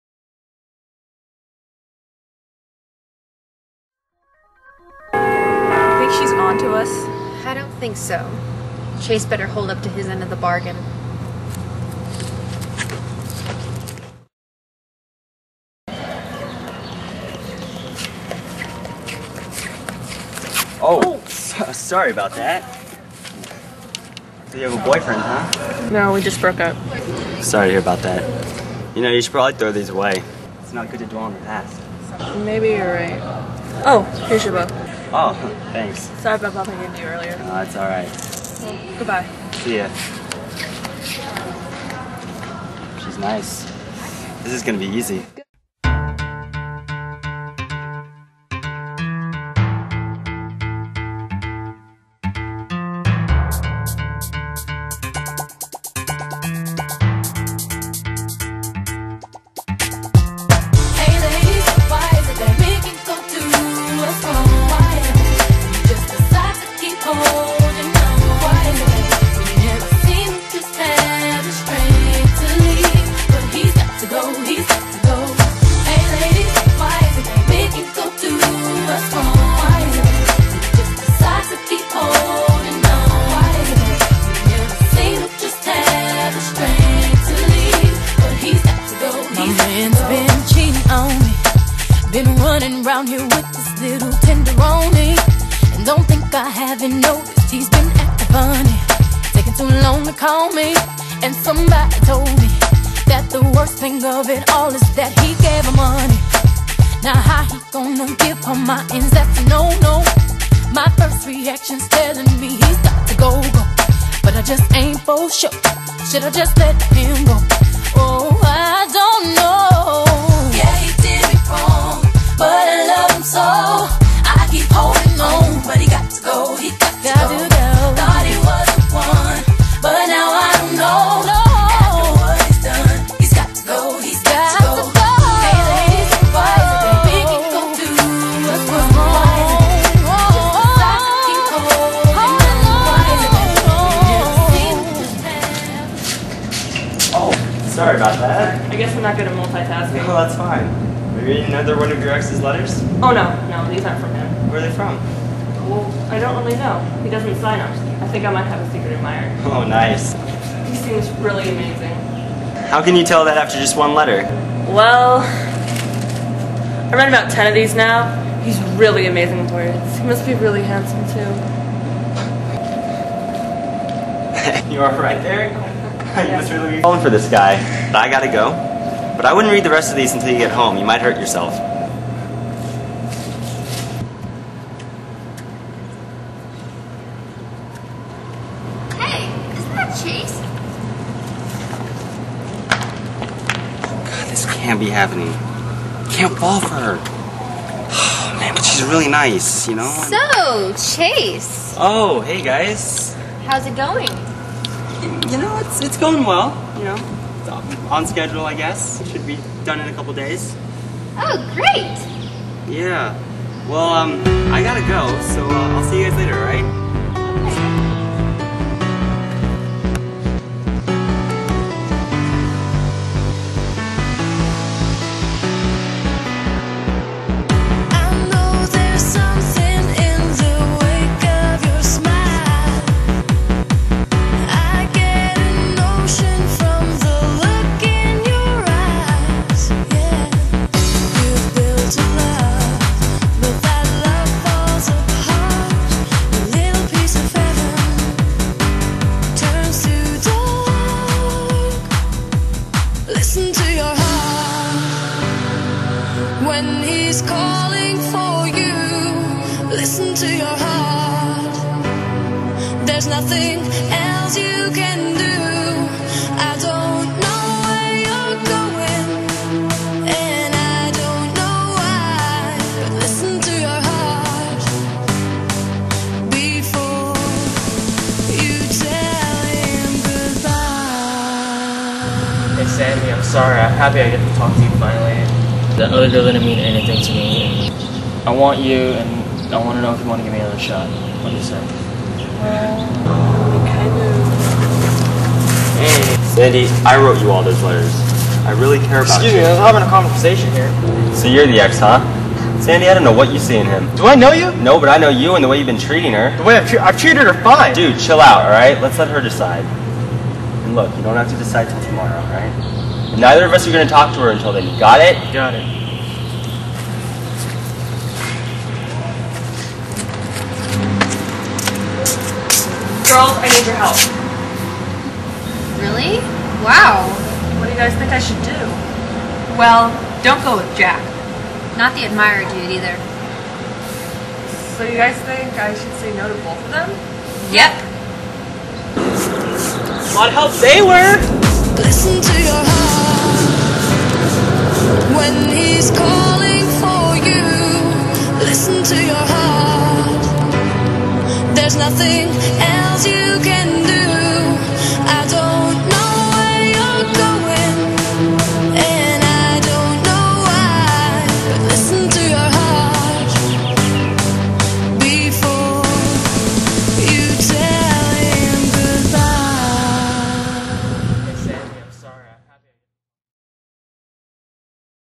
Admirers Part Two - A Student Drama Film - © Jazmyn Whitman
The three-part high-school drama from Animejazmyn continues!  Sometimes even with the best strategy, things don't go exactly as planned.  What happens if you actually let your guard down?  Will your most sensitive emotions be rejected?    Jazmyn Whitman wrote, shot/directed, edited and authored this for advanced video production class.